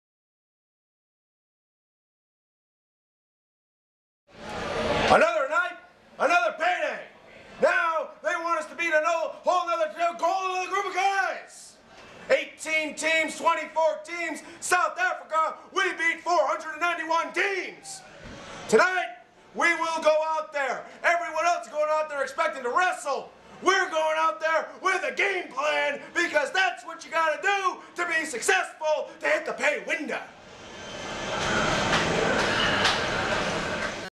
The Devastation Corporation Promo [Tag World Grand Prix 2013]
Chikara DVD May 18, 2013 "Tag World Grand Prix-1st Stage"' Chicago, IL
1. The Baltic Seige (Latvian Proud Oak & Estonian ThunderFrog) vs. The Young Bucks
2. The Spectral Envoy (Hallowicked & Frightmare) vs. Colony: Xtreme Force (deviANT & Orbit Adventure Ant)
3. The Bravado Brothers vs. The Batiri (Obariyon & Kodama)
4. The Daywalkers (Alexandre Barnabus Castle & Mathias Cage) vs. Zero Gravity
5. The Colony (Fire Ant & Green Ant) vs. Los Ice Creams
6. The Devastation Corporation vs. Gary the Barn Owl & Pierre Abernathy
7. Arik Cannon & Darin Corbin vs. Saturyne & Heidi Lovelace
8. Pieces of Hate (Jigsaw & The Shard) vs. Da Soul Touchaz (Marshe Rockett & Acid Jaz)